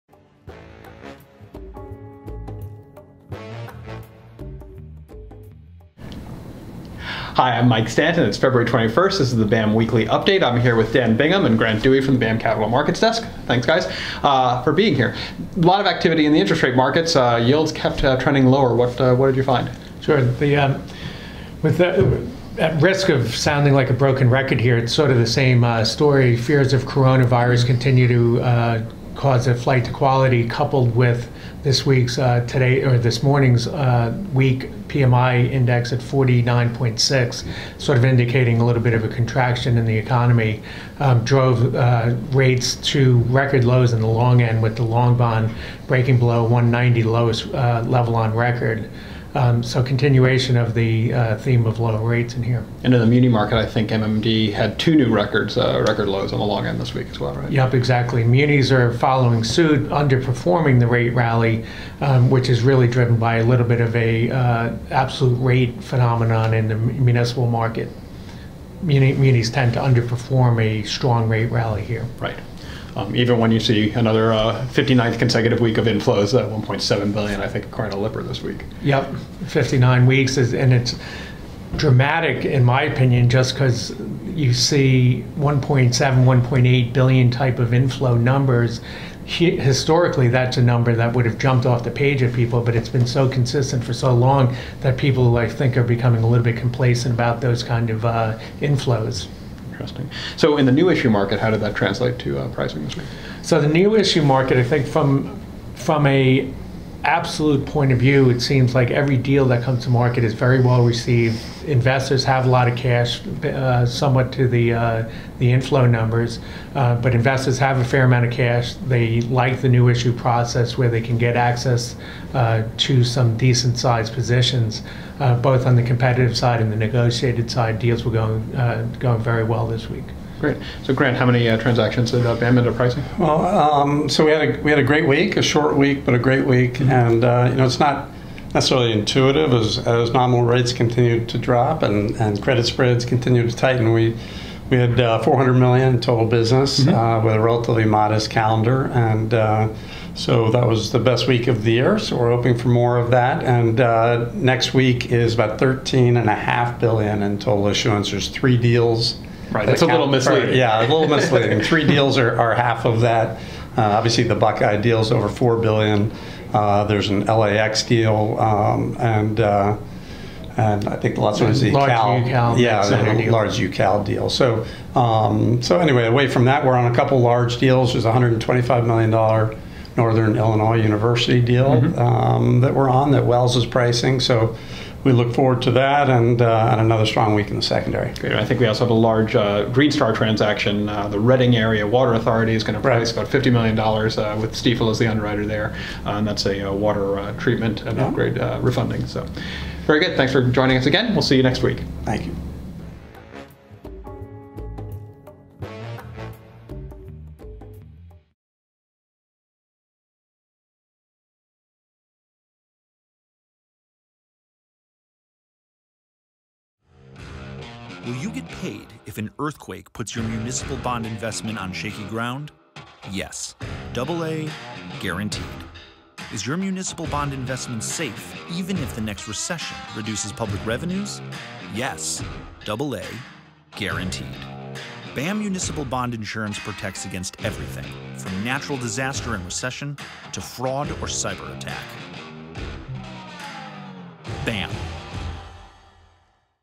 BAM Municipal Market Update - February 21, 2020
Amidst record low yields on Treasuries and municipal bonds, BAM insured approximately $400 million of bonds: The strongest week of the year to date.  The calendar for next week remains strong, led by a $125 million sale for Northern Illinois University and a $50 million BAM GreenStar green bond issue for the Reading Area Water Authority.